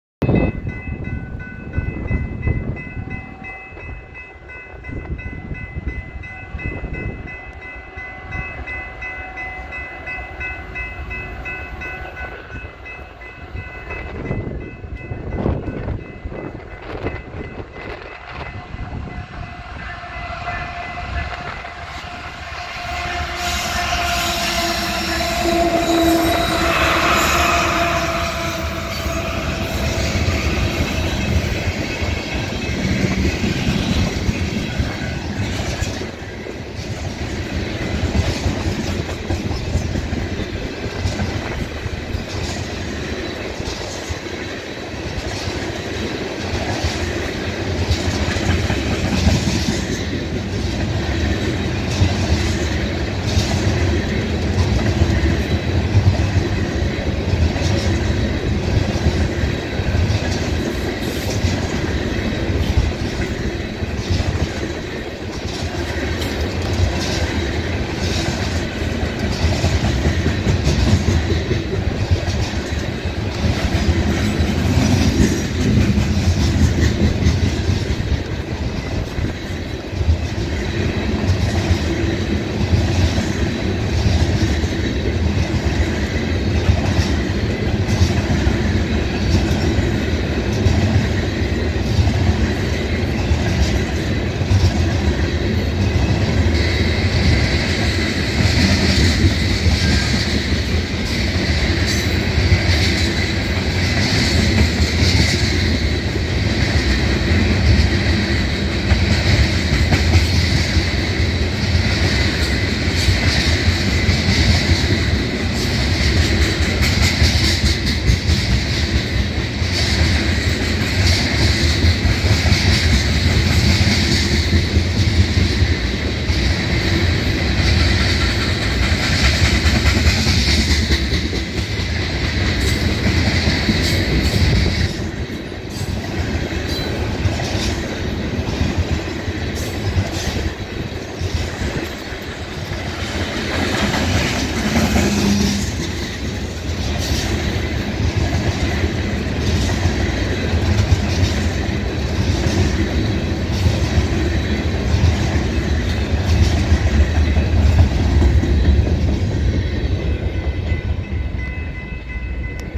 KCS LEAD & NS! CP 528 - KCS 4813E at Milton West - April 23, 2024
Only a couple minutes after the CP 137 Cleared the crossing, CP 528 rolls by with more foreign power! Good thing I filmed at this crossing, as the power would have been blocked by the autoracks of 137 at Steeles. Powering this loaded ethanol train is a Kansas City Southern - Norfolk Southern pair, with KCS Gevo 4813 leading, and Norfolk Southern C6M 4297 trailing second. Not something you see everyday!

Train ID: CP 528 - Eastbound loaded ethanol train from Mason City, Iowa to Albany, New York
Power:
KCS GE ES44AC 4813
NS GE AC4400C6M 4297
F/R

Length: 2 buffer cars, 96 tankers, Total of 98 freight cars and 404 axles
Location: Bronte Street North Level Crossing, Milton, Ontario - Mile 32.81 CP Galt Subdivision
Time: 14h51